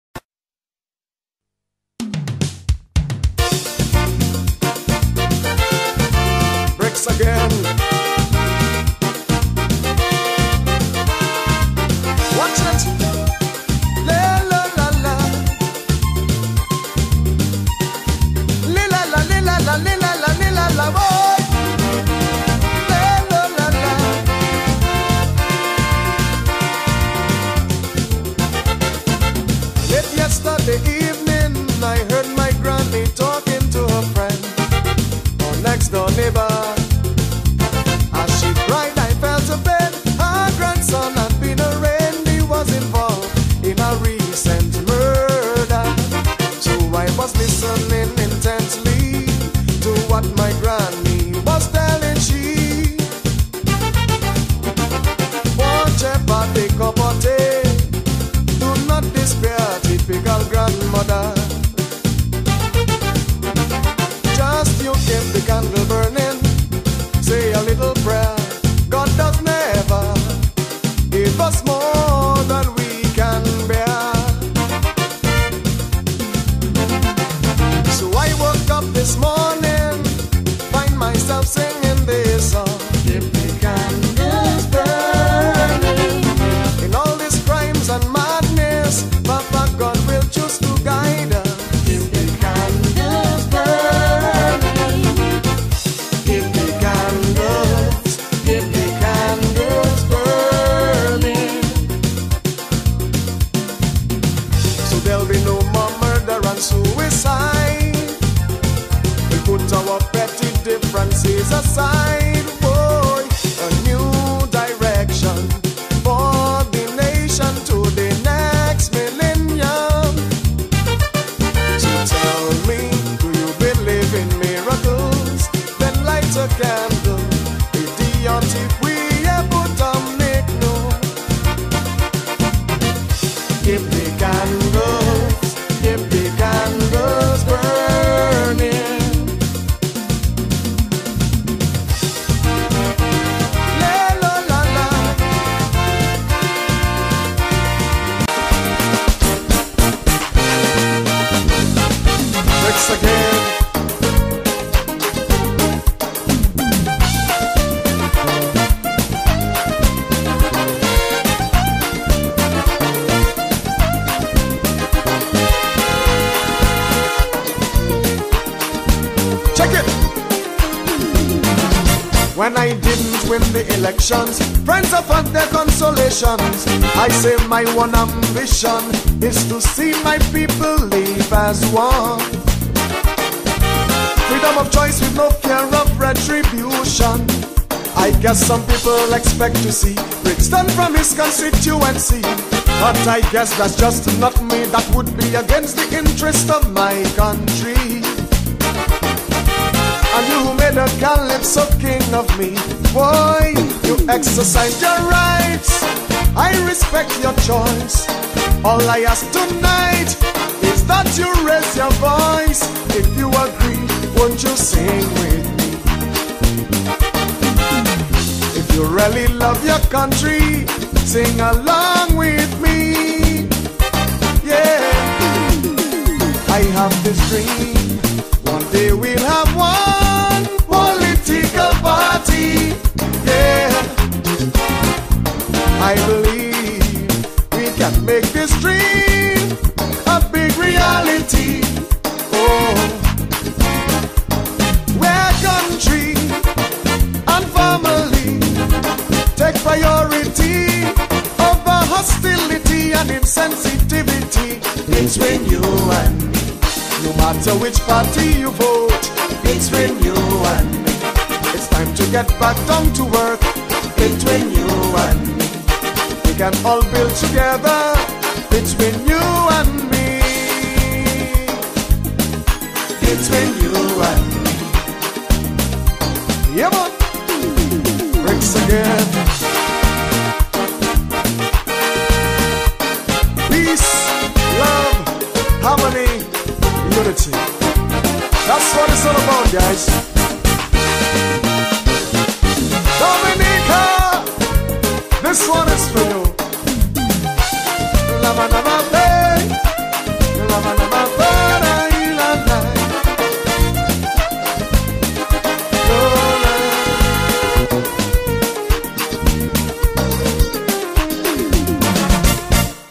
From Brakes to Father John
Former Dominica Calypso King Brancker "The Brakes " John now ordained as Reverend   Brancker John but to me Father Brakes... Two of his more popular hits from the pen of  Dominica Most Prolific Song Writter Pat Aaron